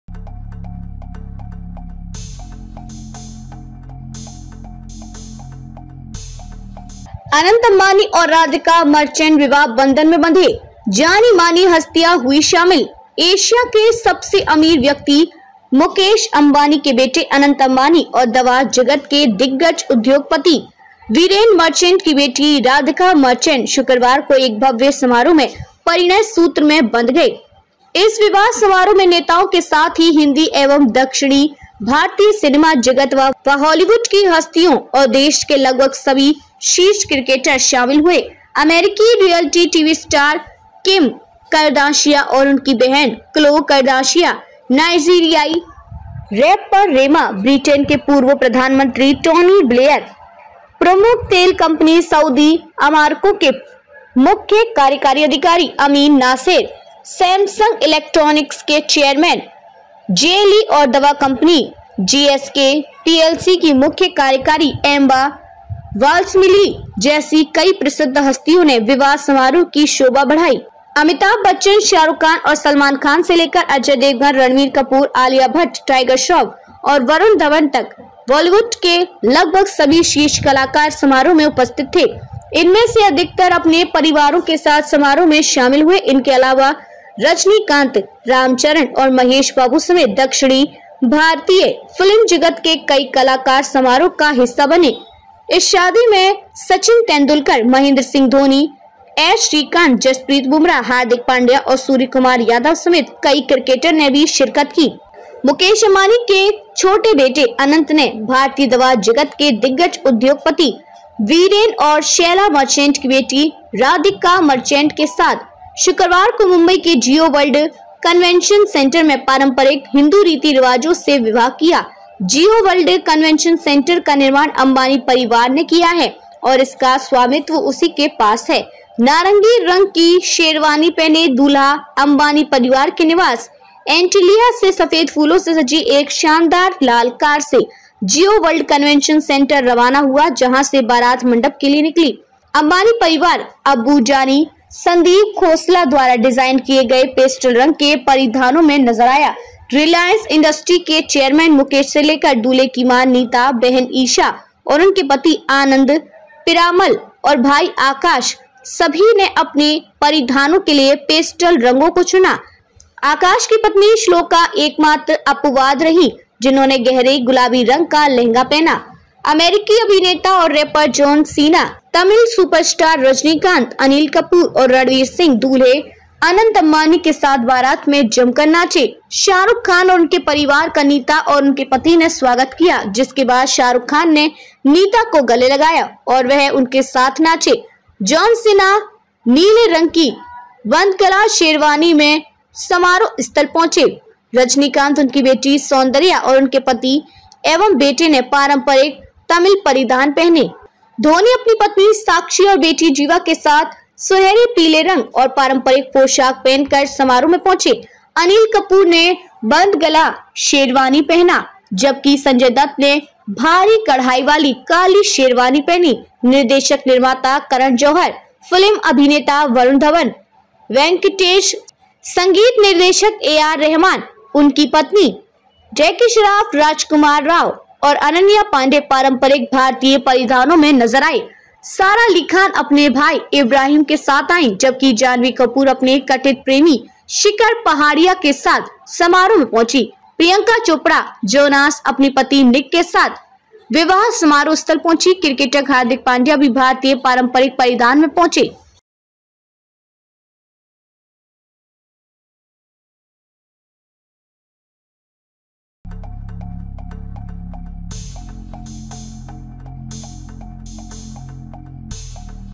बेगानी शादी में अब्दुल्ला दीवाना हो ठुमका वाले घर को आए#FINALLY शादी संपन्न हुई
wedding: एशिया के सबसे अमीर व्यक्ति मुकेश अंबानी के बेटे अनंत अंबानी और दवा जगत के दिग्गज उद्योगपति वीरेन मर्चेंट की बेटी राधिका मर्चेंट शुक्रवार को एक भव्य समारोह में परिणय सूत्र में बंध गए। इस विवाह समारोह में नेताओं के साथ ही हिंदी एवं दक्षिण भारतीय सिनेमा जगत व हॉलीवुड की हस्तियों और देश के लगभग सभी शीर्ष क्रिकेटर शामिल हुए।

दिग्गज हस्तियां जुटीं : अमेरिकी रियलिटी टीवी स्टार किम करदाशियां और उनकी बहन क्लो करदाशियां, नाइजीरियाई रैपर रेमा, ब्रिटेन के पूर्व प्रधानमंत्री टोनी ब्लेयर, प्रमुख तेल कंपनी ‘सऊदी अरामको’ के मुख्य कार्यकारी अधिकारी अमीन नासेर, ‘सैमसंग इलेक्ट्रॉनिक्स’ के चेयरमैन जे ली और दवा कंपनी ‘जीएसके पीएलसी’ की मुख्य कार्यकारी एम्मा वाल्म्सली जैसी कई प्रसिद्ध हस्तियों ने विवाह समारोह की शोभा बढ़ाई।

अमिताभ से शाहरुख तक, सचिन से धोनी तक : अमिताभ बच्चन, शाहरुख खान और सलमान खान से लेकर अजय देवगन, रणबीर कपूर, आलिया भट्ट, टाइगर श्रॉफ और वरुण धवन तक बॉलीवुड के लगभग सभी शीर्ष कलाकार समारोह में उपस्थित थे। इनमें से अधिकतर अपने परिवारों के साथ समारोह में शामिल हुए। इनके अलावा रजनीकांत, राम चरण और महेश बाबू समेत दक्षिण भारतीय फिल्म जगत के कई कलाकार समारोह का हिस्सा बने। इस शादी में सचिन तेंदुलकर, महेंद्र सिंह धोनी, एस श्रीकांत, जसप्रीत बुमराह, हार्दिक पांड्या और सूर्यकुमार यादव समेत कई क्रिकेटर ने भी शिरकत की।shaadi